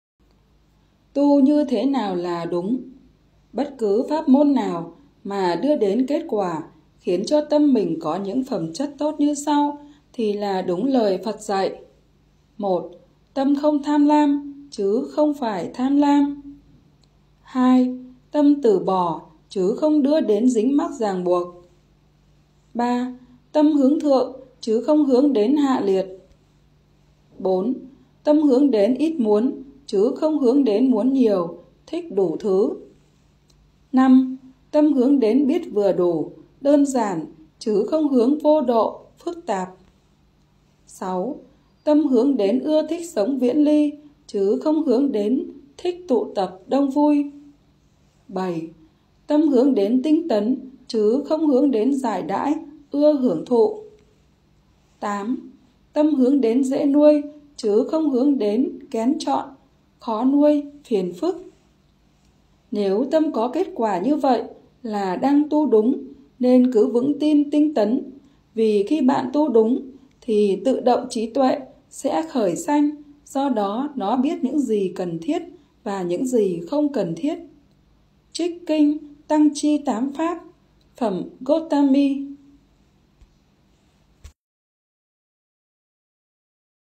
TU NHƯ THẾ NÀO LÀ ĐÚNG | Trích Kinh Tăng Chi Tám Pháp, phẩm Gotami.
TU NHƯ THẾ NÀO LÀ ĐÚNG

 Bất cứ pháp môn nào mà đưa đến kết quả khiến cho tâm mình có những phẩm chất tốt như sau thì là đúng lời Phật dạy:

- Tâm không tham lam chứ không phải tham lam.

- Tâm từ bỏ chứ không đưa đến dính mắc ràng buộc.

- Tâm hướng thượng chứ không hướng đến hạ liệt.

- Tâm hướng đến it muốn chứ không hướng đến muốn nhiều (thích đủ thứ).

- Tâm hướng đến biết vừa đủ (đơn giản) chứ không hướng vô độ (phức tạp).

- Tâm hướng đến ưa thích sống viễn ly chứ không hướng đến thích tụ tập đông vui.

- Tâm hướng đến tinh tấn chứ không hướng đến dãi đãi, ưa hưởng thụ.

- Tâm hướng đến dễ nuôi chứ không hướng đến kén chọn, khó nuôi, phiền phức.

Nếu tâm có kết quả như vậy là đang tu đúng nên cứ vững tin tấn tiến. Vì khi bạn tu đúng thì tự động trí tuệ sẽ khởi sanh do đó nó biết những gì cần thiết và những gì không cần thiết.

Trích Kinh Tăng Chi Tám Pháp, phẩm Gotami.